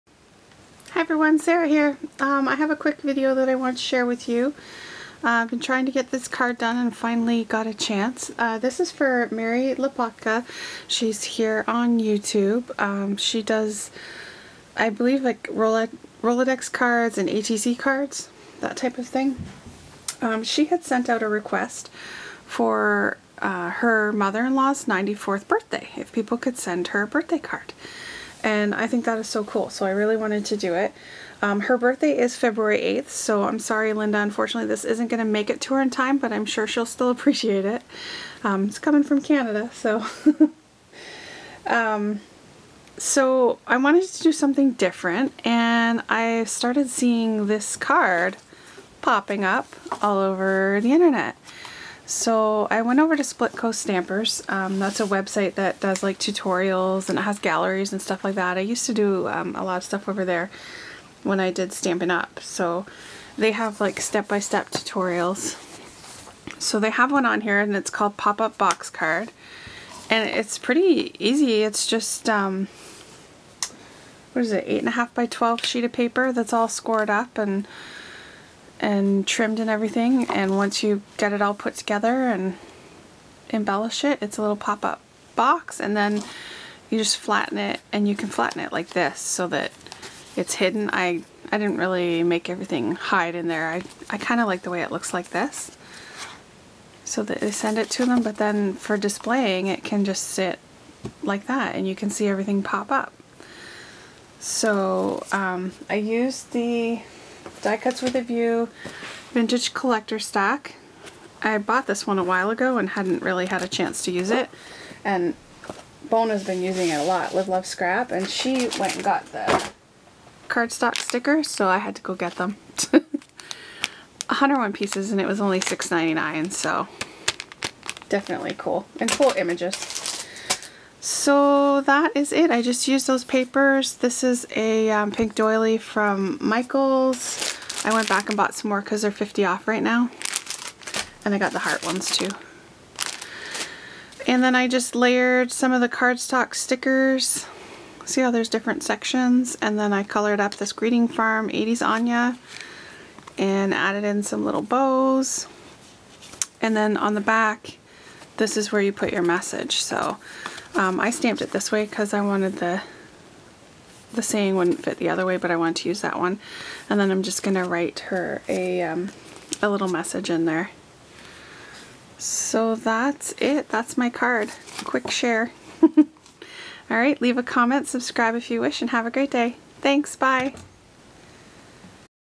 Birthday Card for Linda Lapotka's Mother in Law
Here is a birthday card I made for Mary's 94th birthday which is Feb 8th.
Tutorial is here on splitcoaststampers...